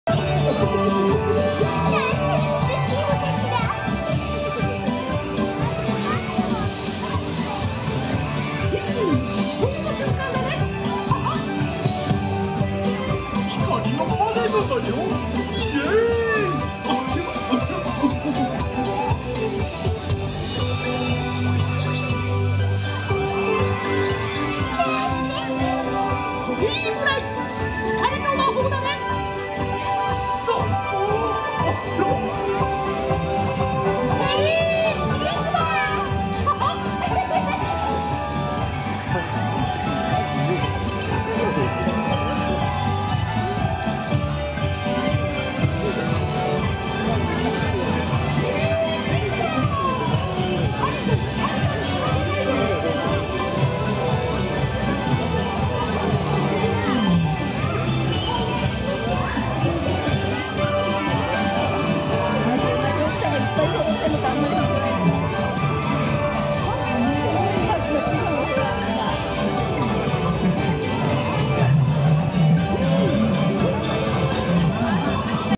☆エレクトリカルパレード☆２０１１．１２．２０ ①
お馴染み！東京ディズニーランドの☆エレクトリカルパレード☆クリスマスヴァージョン！